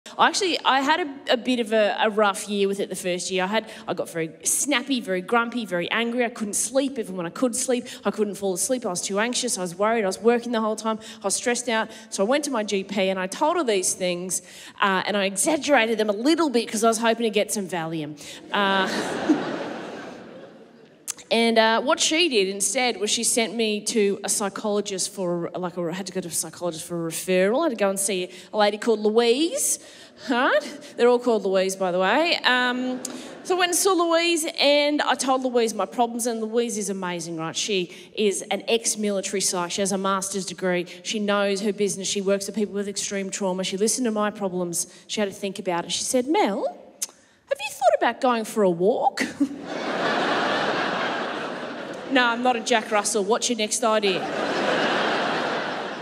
It's a tough job 🙃
Mel Buttle talks about how hard being a new mum can be.

Melbourne International Comedy Festival favourite Lizzy Hoo hosts the biggest night of Australia's comedy calendar. The Gala is a powerful force supporting Oxfam's mission to eliminate poverty and tackle global inequality.